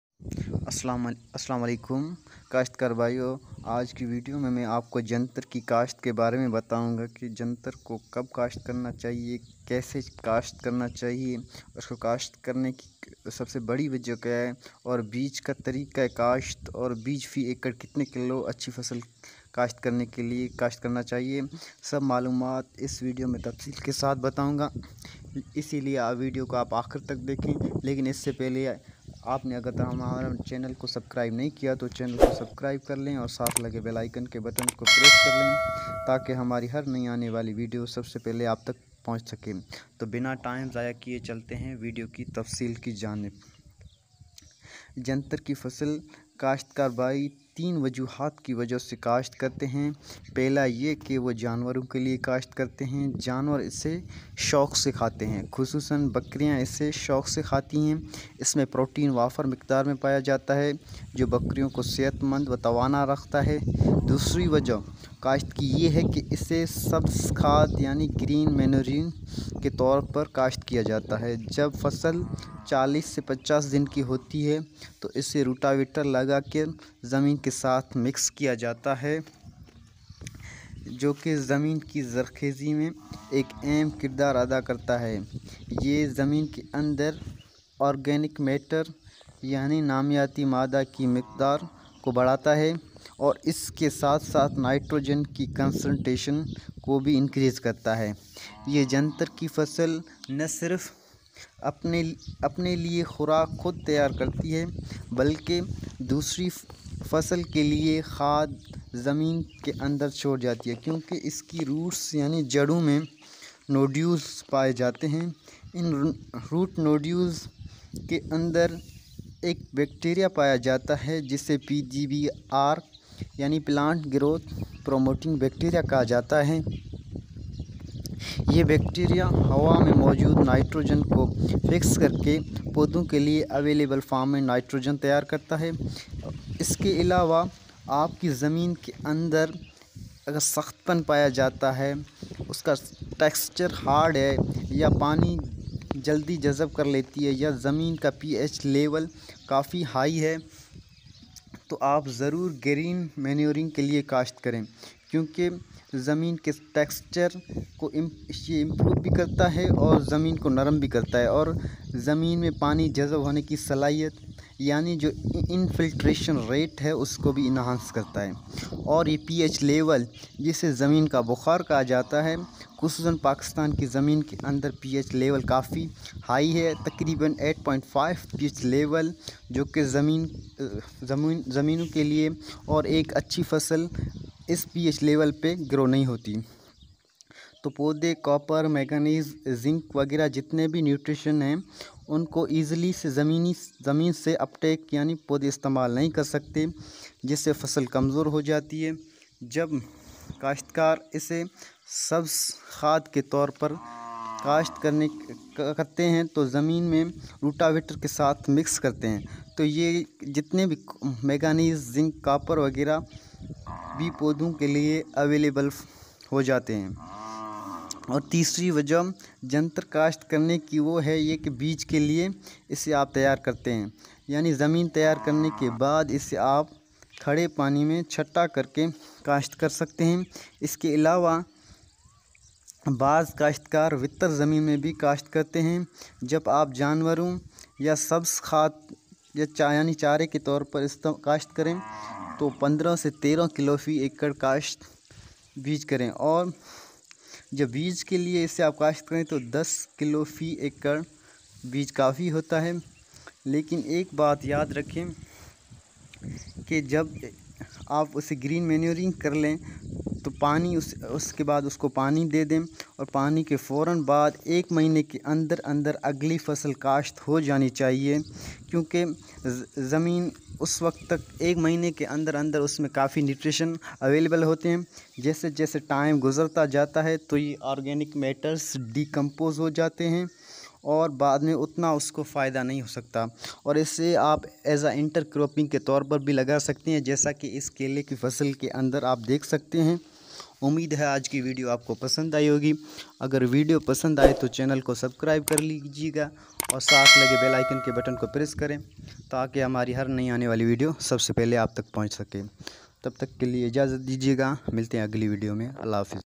Janter ki kasht|Janter farming | sesbiania cultivation in pakistan|Green manuring crop|جنتر کی کاشت
Janter is best crop for green manuring. In
 the roots of janter, root nodules occur, in which PGPR |Plant growth-promoting rhizobacteria found and this bacteria fixed the atmospheric nitrogen in the root zone of plants that plant can easily uptake the nitrogen.
Green manuring practice enhances the water holding capacity and removes the soil hardpen that plants can easily grow.
The pH level of Pakistani soil is high more than 8.5 which is very high and plants in these conditions can not uptake copper , manganese sulphur and other essential nutrients but due to this practice soil pH decrease and plants easily uptakes these nutrients.
This process of cultivation of Janter crop grow for greenmanuring and rotavate and mixed them in the soil with rotavator.
Animals eats the janter crop keenly specially small animal like goats.
janter is also rich source of protein that's why it makes healthy and fit to the animals.